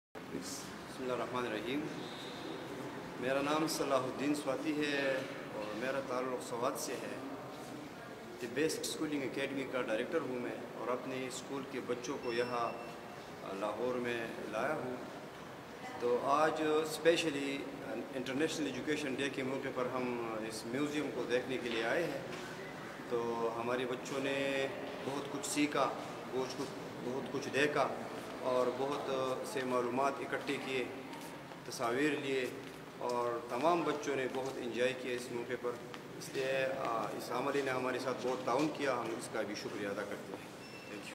Mr. Salah ud Din Swati sharing experience about Lahore Museum.
On the eve of International Education Day, Mr. Salah ud Din Swati and students from The Best Schooling Academy, Swat visited Lahore Museum.
He is sharing experience about Lahore Museum.